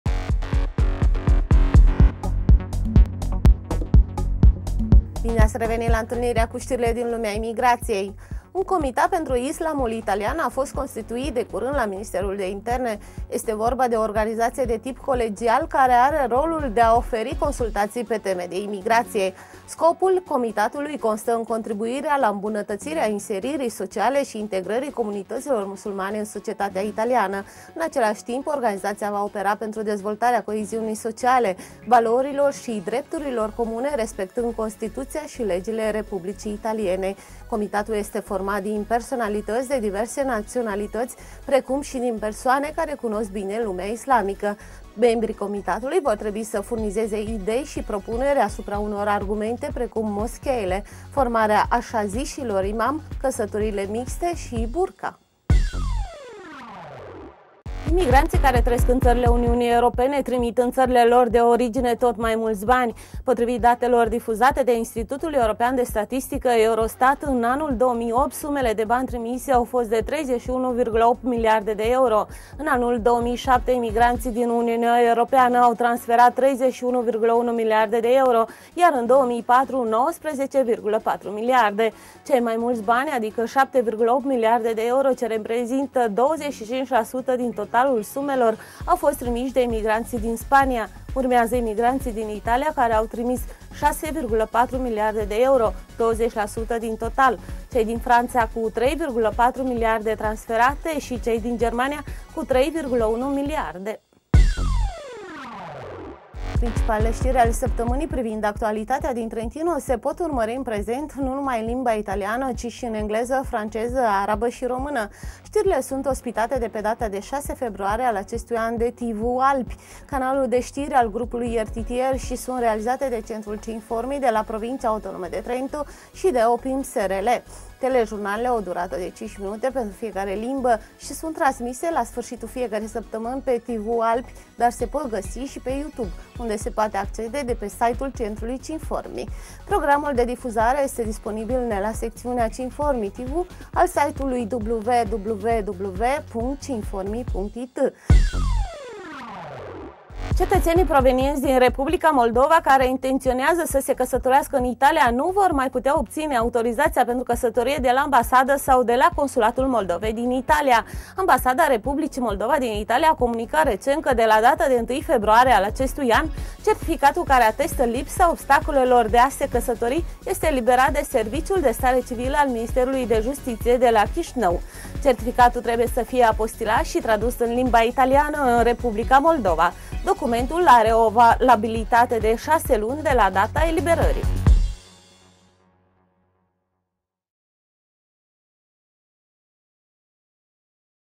Cinformi Tv - News 17/02/2010 română
Notizie flash della settimana sull'immigrazione. Cinformi news è un video registrato a Trento in Via Zambra n. 11 dalla Provincia autonoma di Trento per il sito versione elettronica dalla rivista Cinformi news supplemento de Il Trentino. In redazione Pierluigi La Spada (coordinamento editoriale), Anna Eccher, Andrea Cagol (coordinamento giornalistico), Magdalena Luca, Anna Lanfranchi, Azim Koko. Direttore responsabile de Il Trentino Giampaolo Pedrotti.